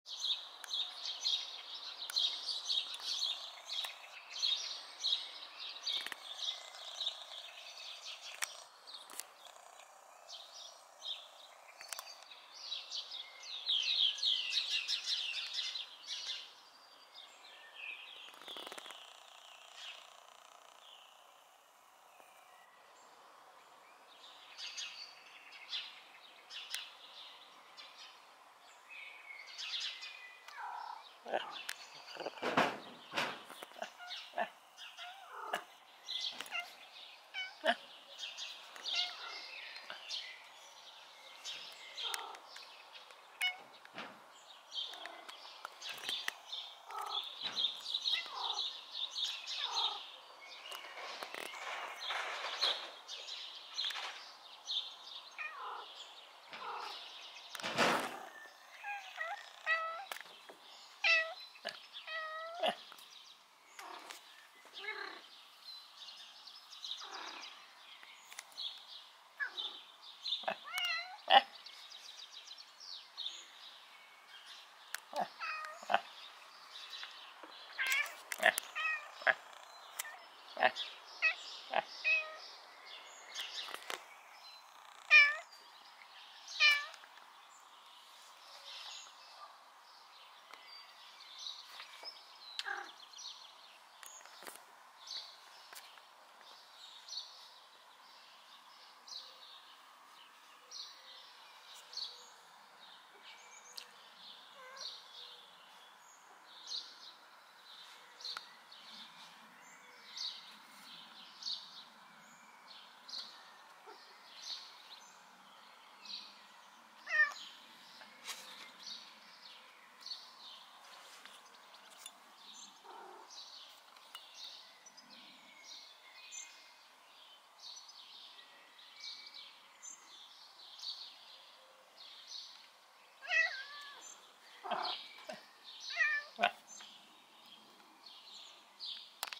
2023-05-27 LJUBLJANA Doma CARNIVORA Felis catus DOMAČA MAČKA (Bunkica, Tigrček in Lumpa)
... kaj mački počno med tretjo in četrto uro zjutraj?